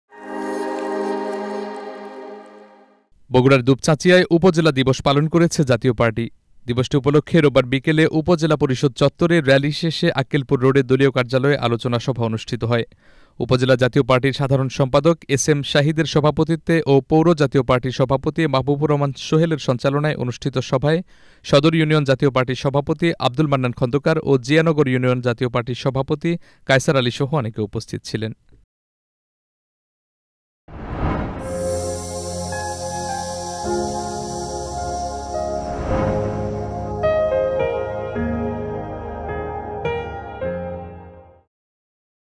বগুড়ার দুপচাঁচিয়ায় উপজেলা দিবস পালন | Channel S News | Channel S 2022
বগুড়ার দুপচাঁচিয়ায় উপজেলা দিবস পালন করেছে জাতীয় পার্টি। দিবসটি উপলক্ষে রোববার বিকেলে উপজেলা পরিষদ চত্ত্বরে র‌্যালি শেষে আক্কেলপুর.

1: Ajker Desh Jure News :
2: Channel S Rater Songbad News :